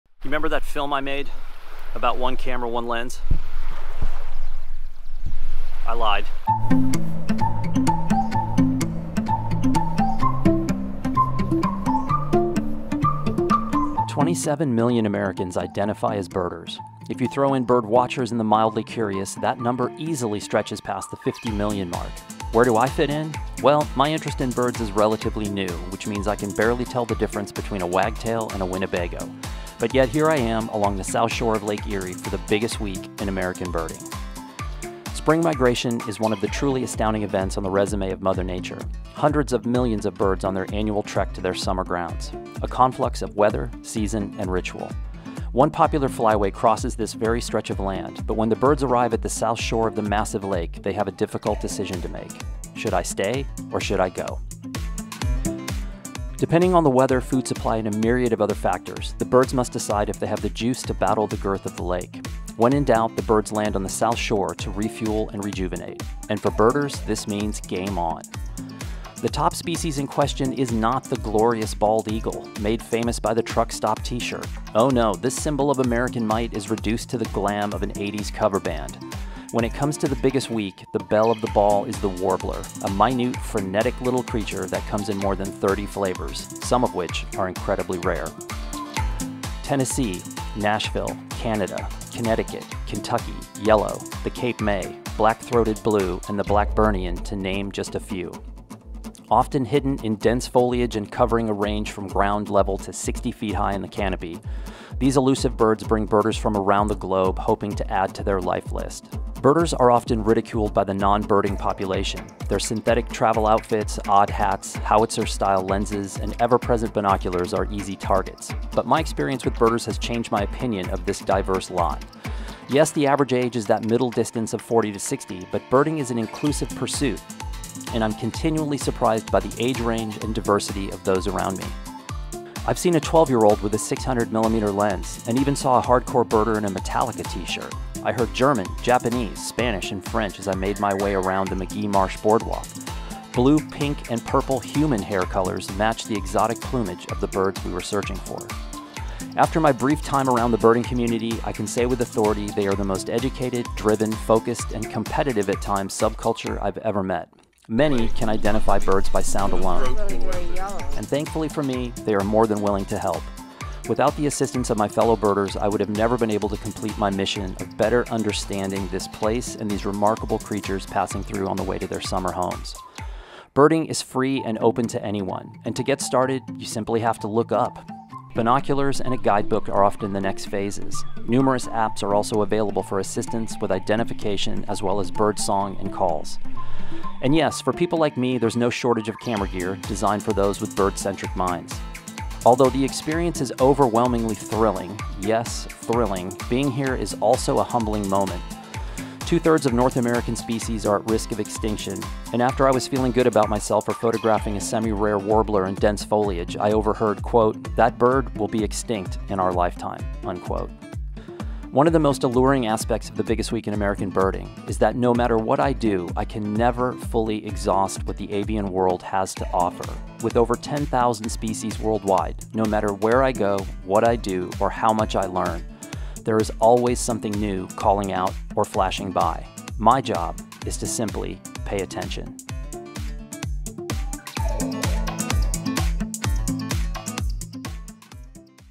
From The Van With Dan | Episode Twelve: The Biggest Week in American Birding
What do birding and photography have in common? More than you'd think.

Birders and photographers alike find joy in the thrill of discovery, whether it's spotting a rare species or capturing an elusive shot. Both offer a unique perspective on the world, encouraging us to slow down, pay attention, and marvel at the beauty that surrounds us every day.

In this episode of FromTheVanWithDan, join Dan Milnor as he explores the intersecting worlds of birding and photography during the biggest week in birding at Lake Erie. Experience the thrill of spotting a rare bird species, the anticipation of capturing the perfect shot, and the sheer joy of being one with nature.

Whether you're a seasoned birder, an aspiring photographer, or simply a nature enthusiast, grab your binoculars, pack your camera, and join us on this exciting adventure!